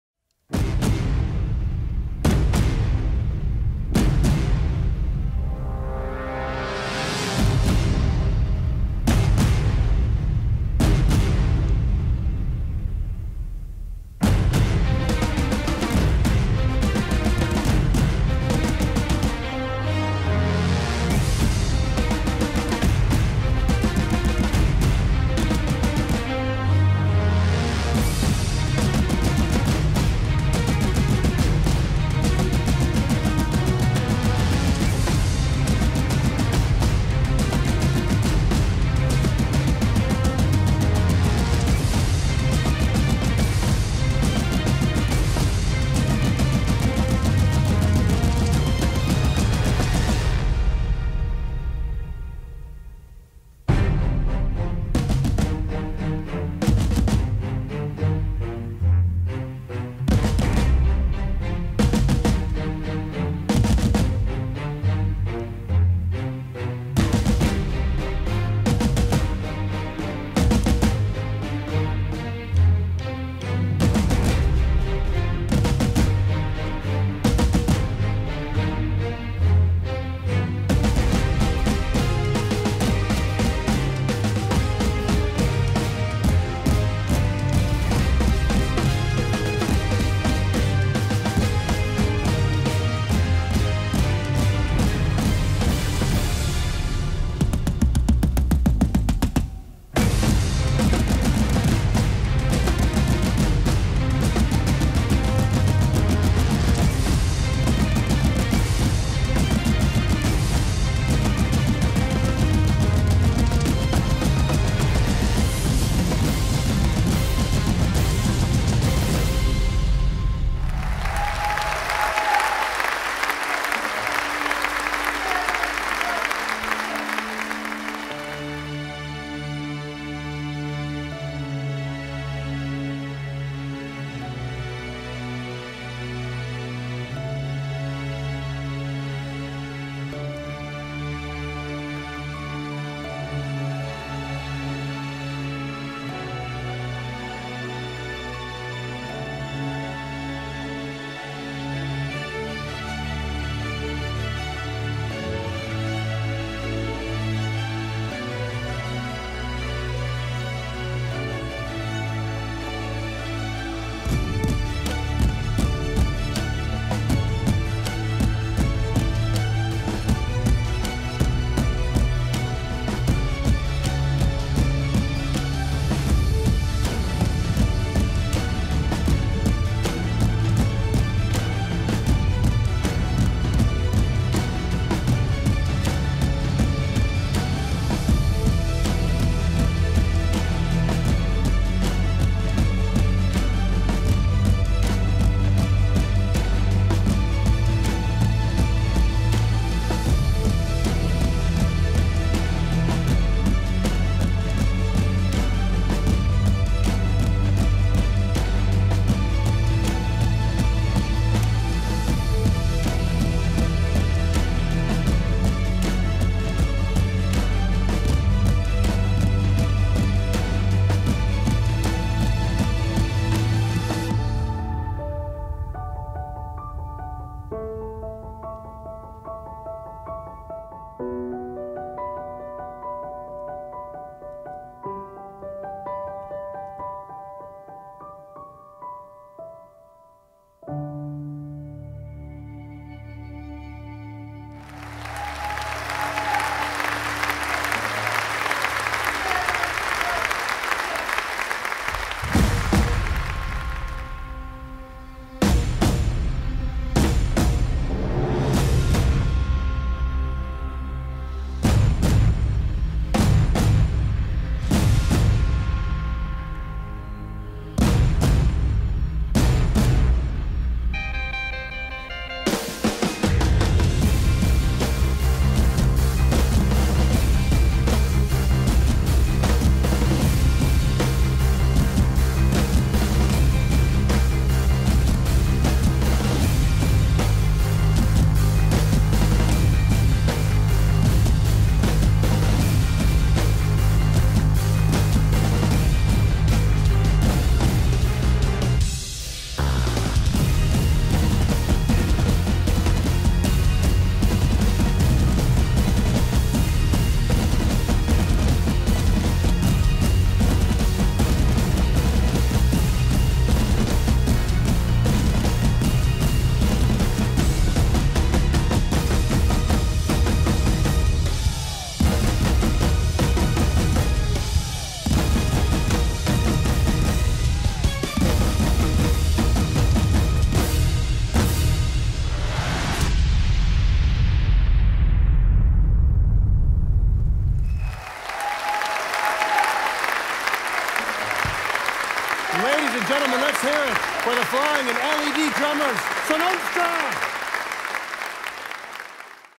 FlyingDrummers ® at Liberty Global for Neoc
[Lightcontrol by The Art of Light]

Compilatie van het showblok tijdens de Tech Summit 2016 in De Ziggo Dome.